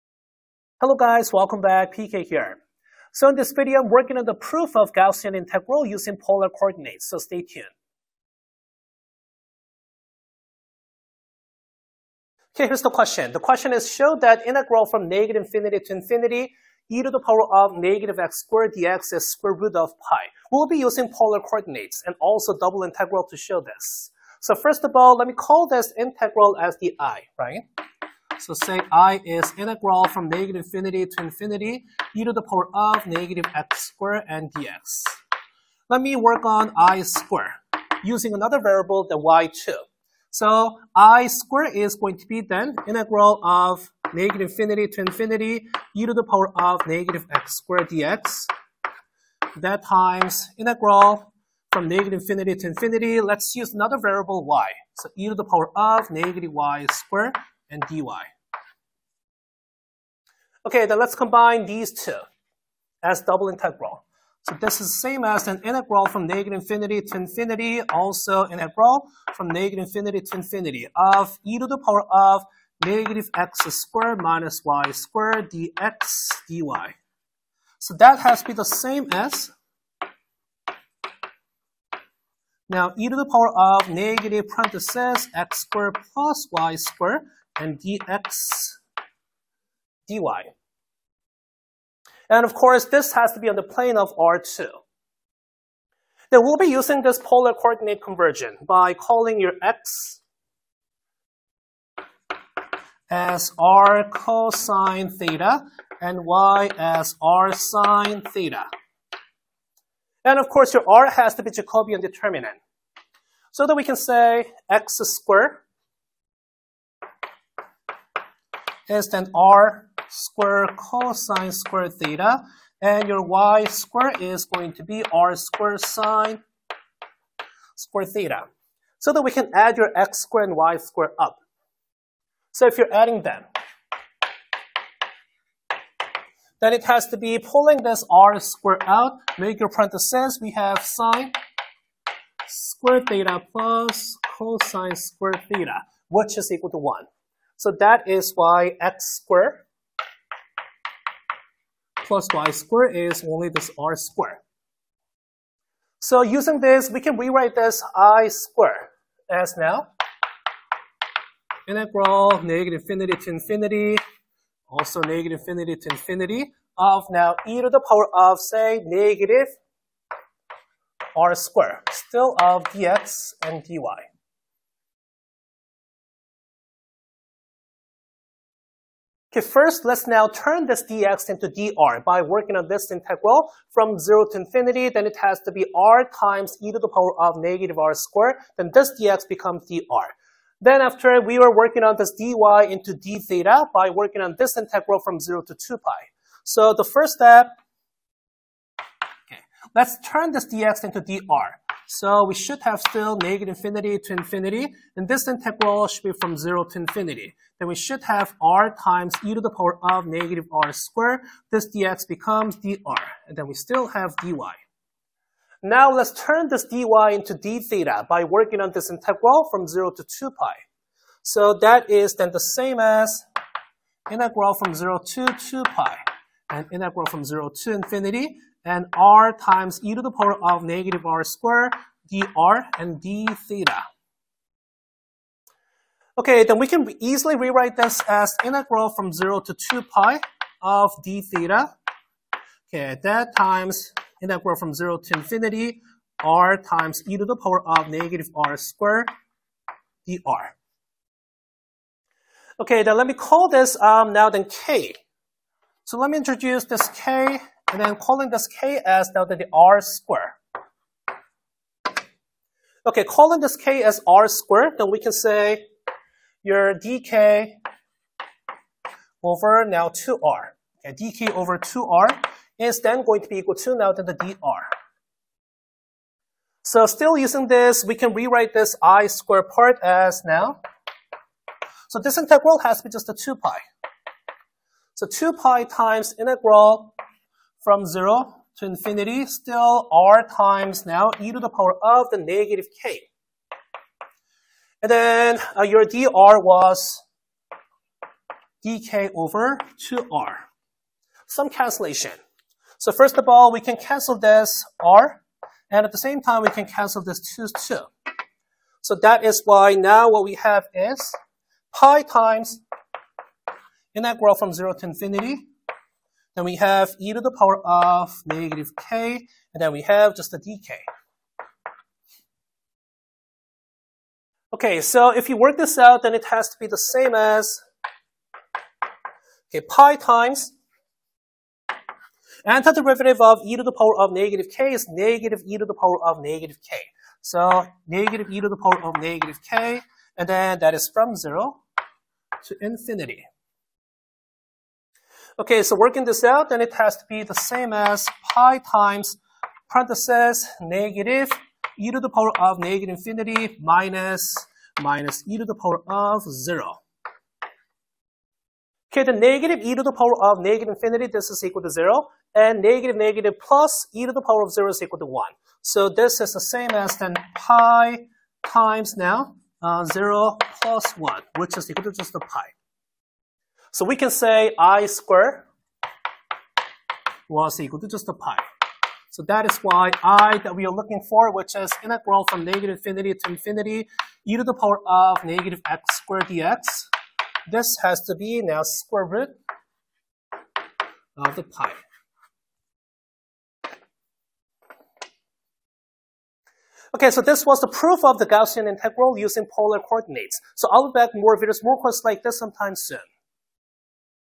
The Most Exciting Integration Weapon
In this video, I am working on the simple proof of Gaussian integral using polar coordinates.